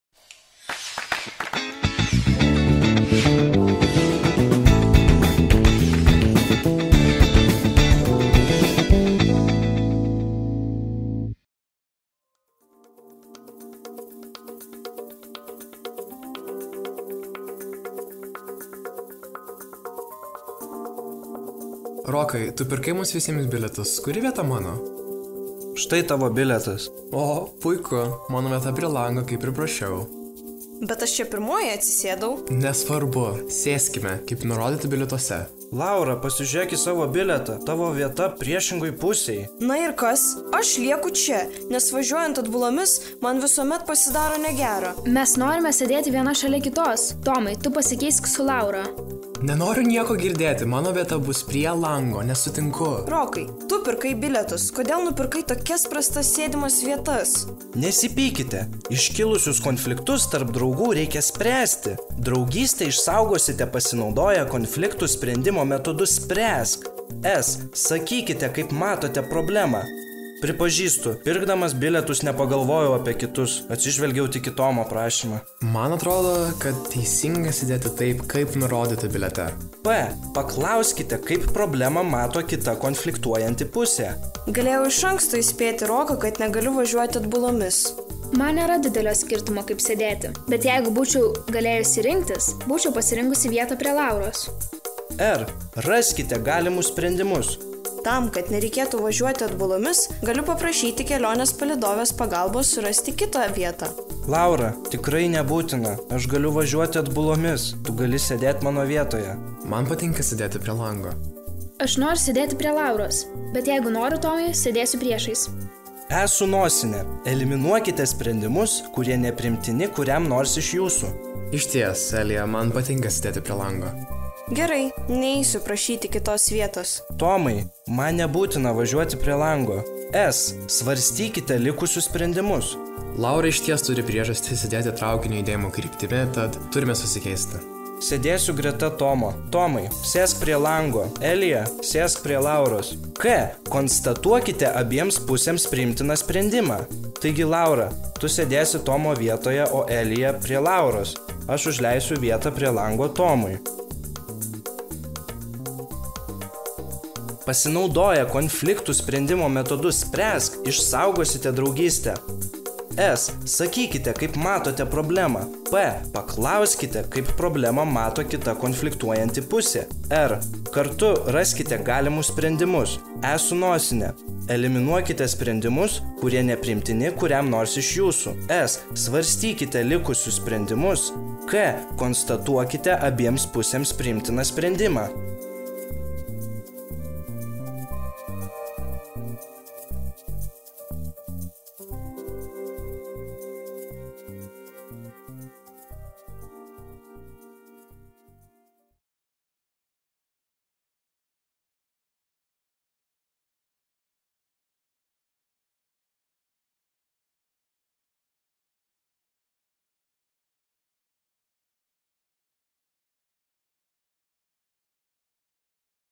SPRĘSK. Konfliktų valdymo metodas. LIONS QUEST programa. Mokytojams skirta mokomoji medžiaga.
LIONS QUEST LIETUVA ir „Tylus kinas“ išleido animacinių filmukų seriją, kuri yra pagalba mokytojams ir tėvams. Filmukai padės paaugliams geriau suprasti savo emocijas ir ugdytis socialinius–emocinius įgūdžius.

Šis filmukas rodo kiek ir kokių reikia žingsnių, kurie padės spręsti kylančius tarpusavio konfliktus mokykloje, klasėje ir namuose.

Filmukas tinka paaugliams, mokytojams ir tėvams.

„Tikimės, kad filmukai neatstos pamokos ir paties mokinių–mokytojo santykio, o bus kaip pagalbinė priemonė mokytojams ir tėvams supažindinant vaikus su socialinių ir emocinių įgūdžių raiška realiame gyvenime“, - sako dr. D. Šukytė.

Sveikatos stiprinimo fondo finansuojamas LIONS QUEST LIETUVA projektas.